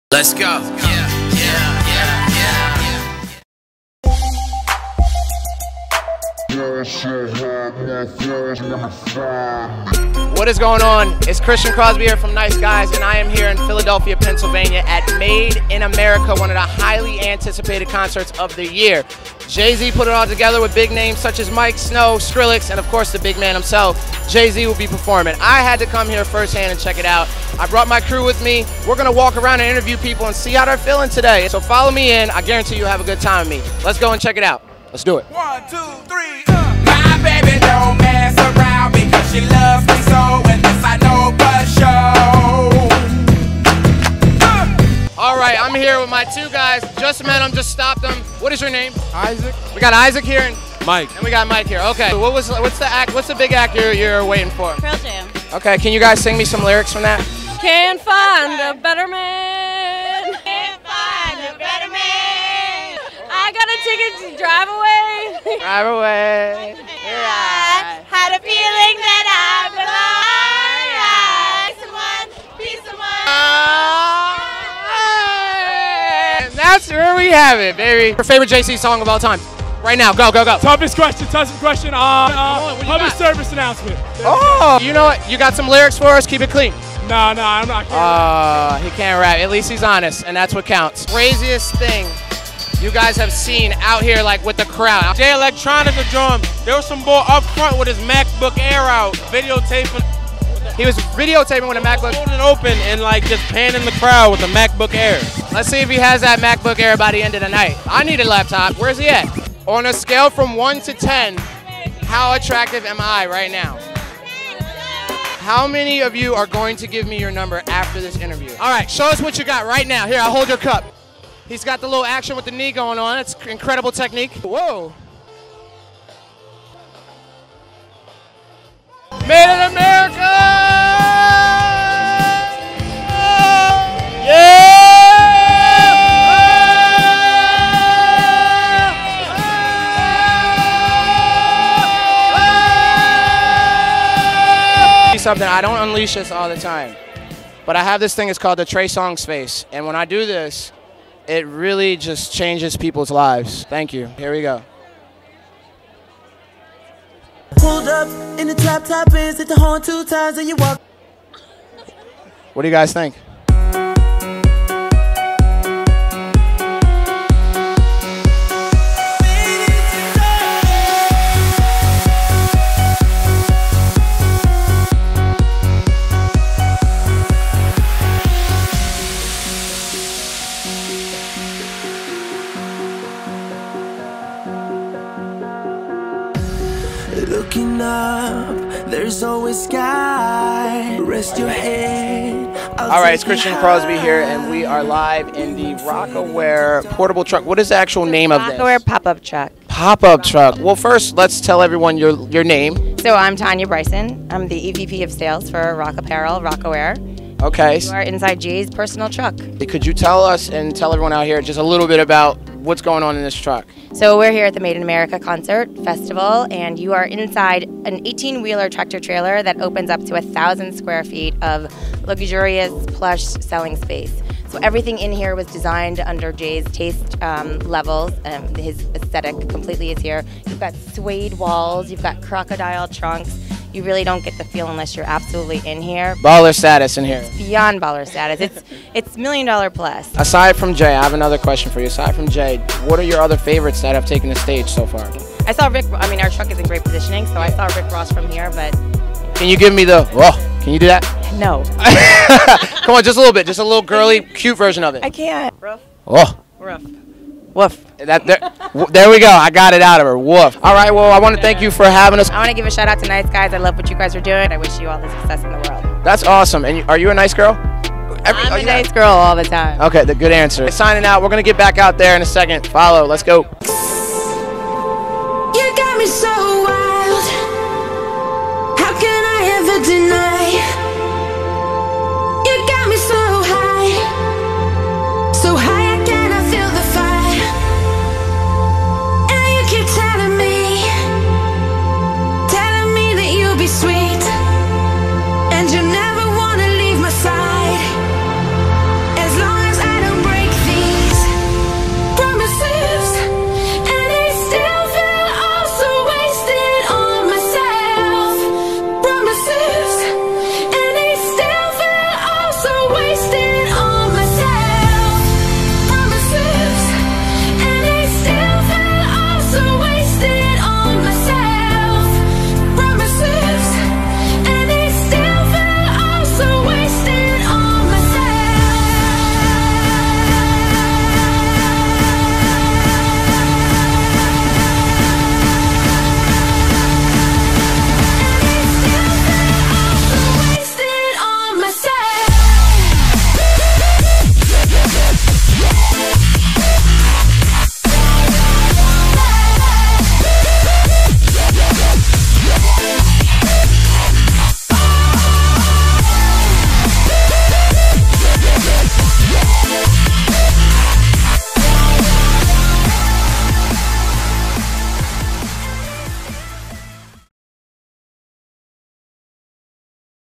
Christian Crosby - Nice Guys x Made In America
Check out Christian Crosby joined by his Nice Guys crew documenting one of the biggest concerts of the year, "Made In America" in Philadelphia, Pennsylvania. Rocawear EVP of Sales, Tanya Bryson shows the Nice Guys crew some behind the scenes love in Jay-Z's personal pop-up truck. From interviews, dancing with random fans, to live performances from the worlds most successful artists, there is never a dull moment in this video. Enjoy!